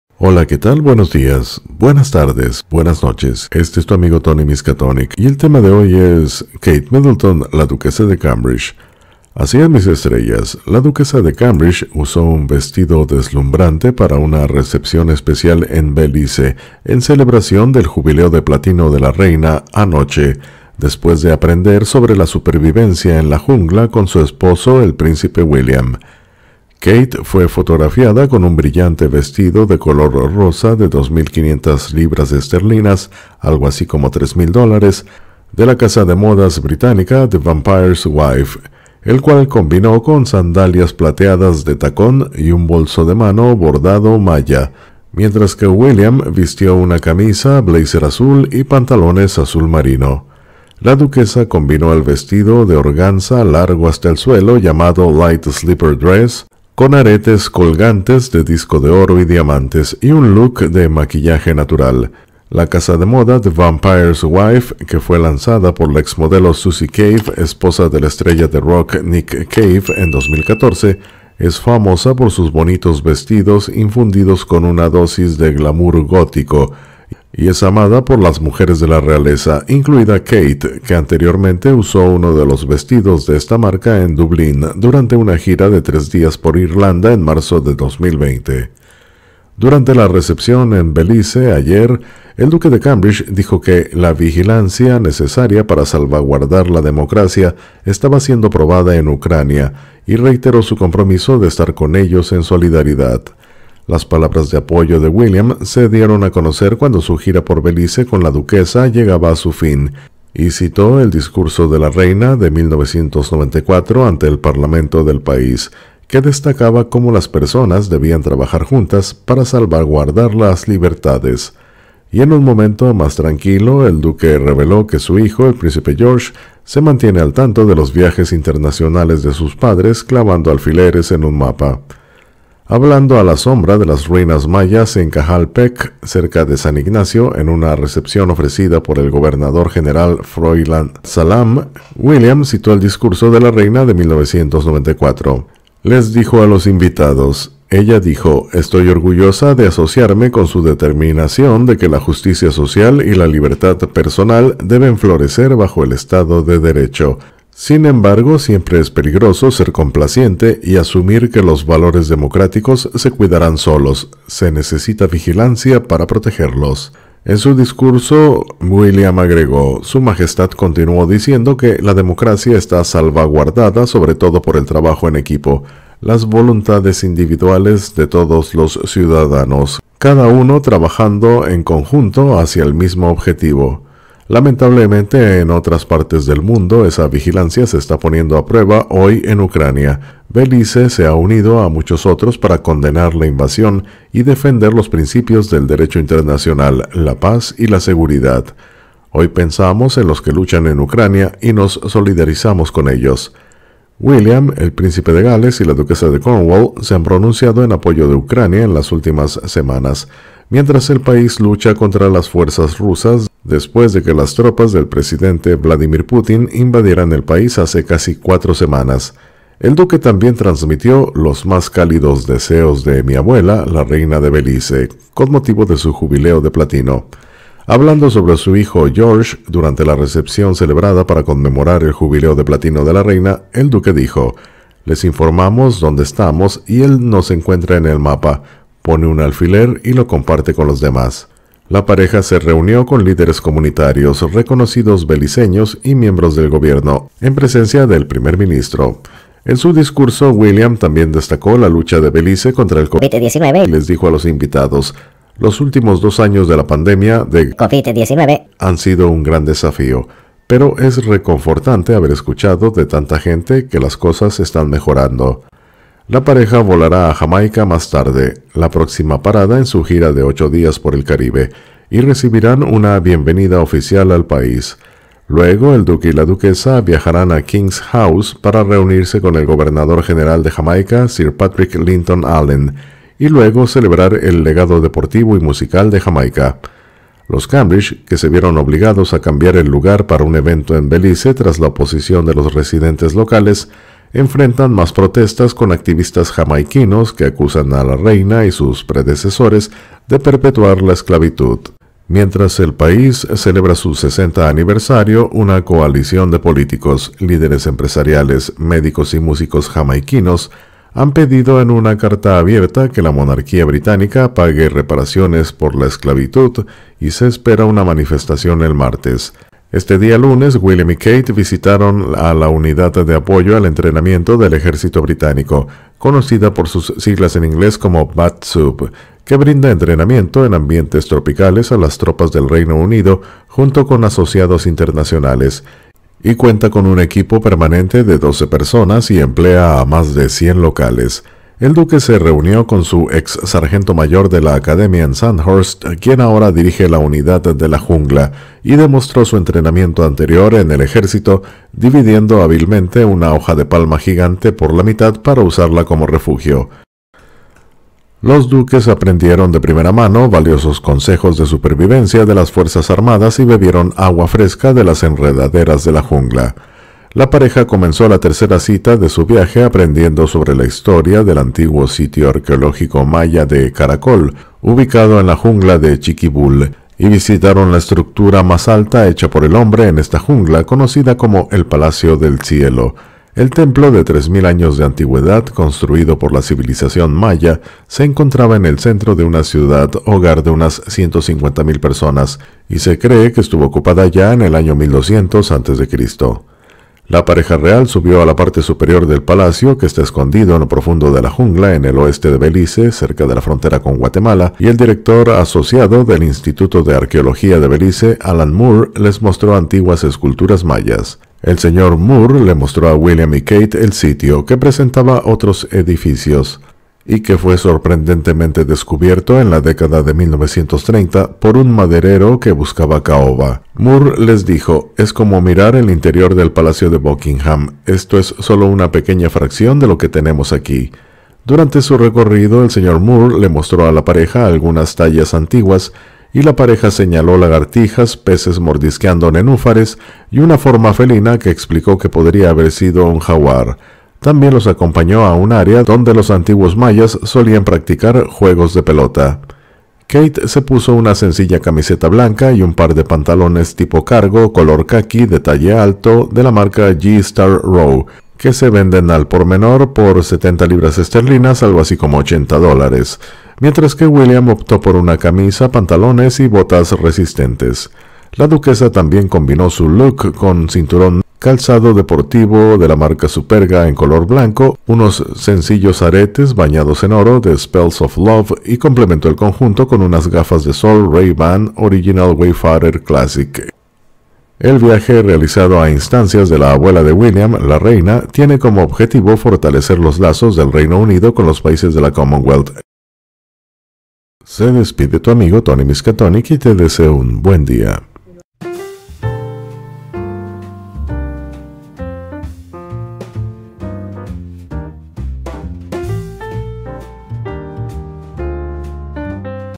KATE MIDDLETON ILUMINA LA SELVA CON SENSACIONAL VESTIDO ROJO. GIRA DE LOS CAMBRIDGE EN BELICE
Únete a este canal para acceder a sus beneficios:
El duque y la duquesa de Cambridge emprenden un viaje de ocho días para conmemorar el jubileo de platino de la reina
La pareja real visitó la Unidad de Apoyo al Entrenamiento del Ejército Británico y aprendió consejos de supervivencia en la selva tropical
William y Kate luego mostraron un gusto por la moda impecable mientras se vestían con un impresionante atuendo de noche.
Ayer visitaron el antiguo sitio arqueológico maya de Caracol ubicado en lo profundo de la selva beliceña.
Sigue la visita a la finca de cacao de gestión familiar y las lecciones sobre el intrincado proceso de elaboración del chocolate ayer.
La primera parte de su gira tuvo que ser cancelada por las protestas por los planes de aterrizar un helicóptero en el campo de fútbol local.